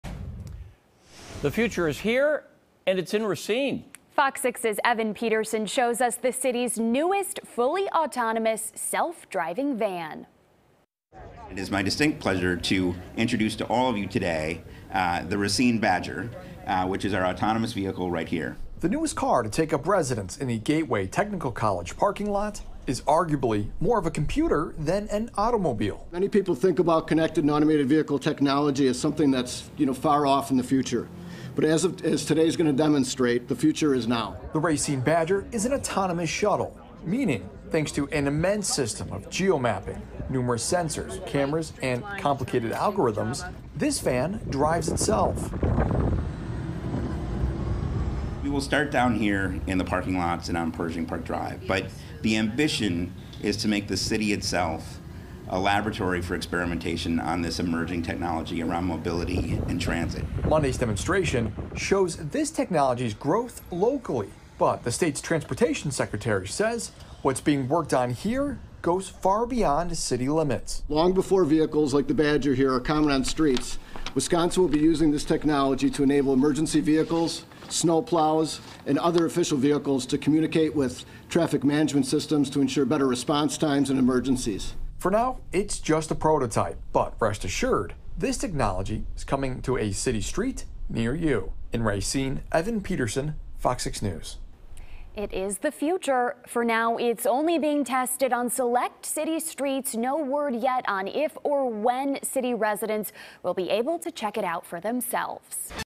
Badger Racine's new driverless vehicle | FOX6 News Milwaukee
The future is here in Racine, where officials unveiled the "Badger," a fully autonomous, all-electric, zero-emission shuttle.